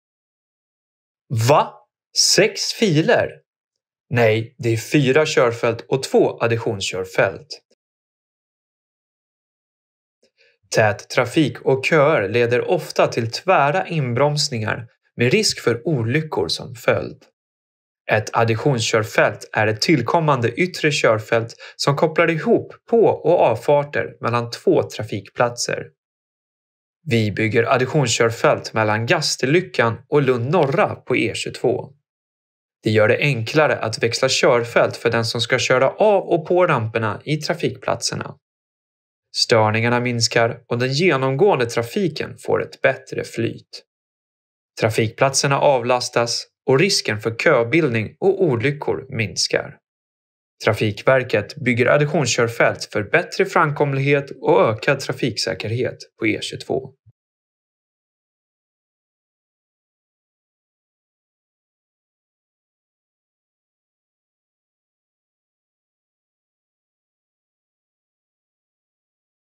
Vi bygger additionskörfält mellan trafikplatserna Gastelyckan och Lund Norra på E22 | Trafikverket
Trafikverket bygger additionskörfält för bättre framkomlighet och ökad trafiksäkerhet på E22 mellan Gastelyckan och Lund Norra i Skåne län. Additionskörfältet är ett tillkommande yttre körfält som kopplar ihop på- och avfarter mellan de båda trafikplatserna. Det gör det enklare att växla körfält för den som ska köra av och på ramperna i trafikplatserna. Trafikplatserna avlastas och risken för köbildning och olyckor minskar.

Syntolkning:
Filmen är en animering och visar bilder av hur Trafikverket bygger additionskörfält mellan trafikplatserna Gastelyckan och Lund Norra. Animeringen visar också hur additionskörfältet kopplar ihop på- och avfarter mellan de båda trafikplatserna, att det blir enklare att växla körfält för den som ska köra av och på ramperna i trafikplatserna. Genom additionskörfältet kommer trafikplatserna att avlastas och risken för köbildning med olyckor som följd kommer att minskas.